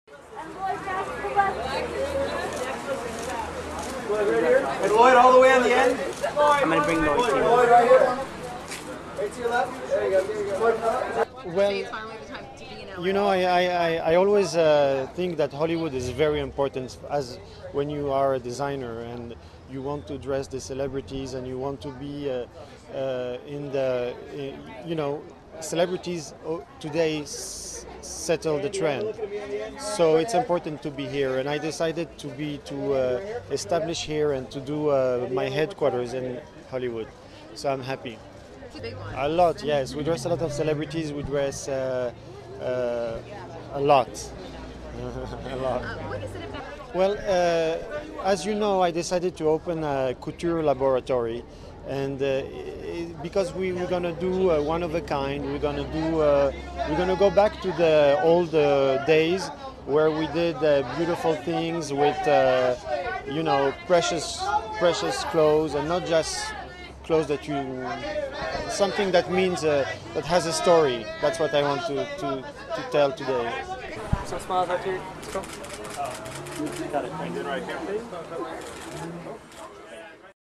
Lloyd Klein Celebrity Launch Party - Lloyd Klein
World Renowned Couturier Lloyd Klein Opens Flagship Retail Store, Showroom, and Studio in Los Angeles With a Celebrity Launch Party - Beverly Hills, CA November 14, 2006